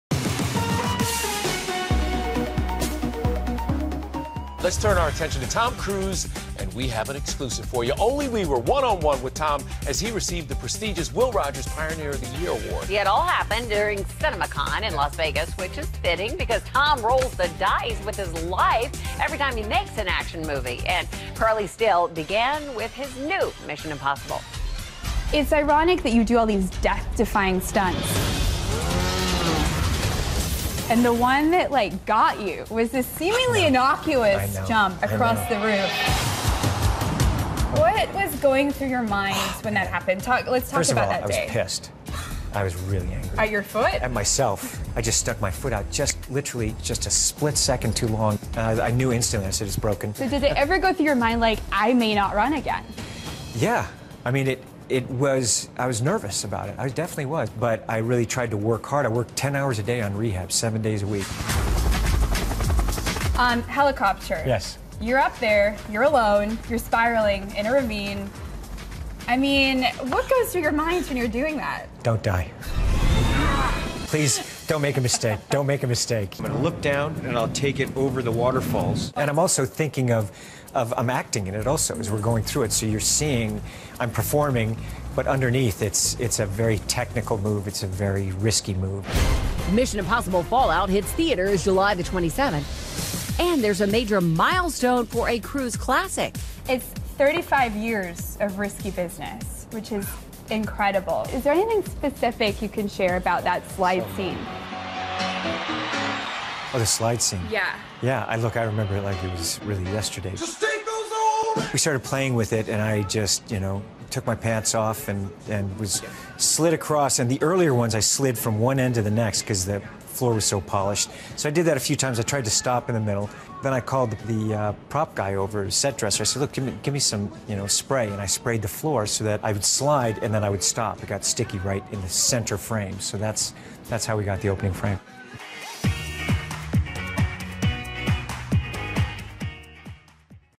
Tom Cruise Admits He Was 'Pissed' Following 'Mission: Impossible' Injury (Exclusive)
Cruise opens up about breaking his ankle while performing a stunt on the set of 'Mission: Impossible -- Fallout,' which hits theaters on July 27.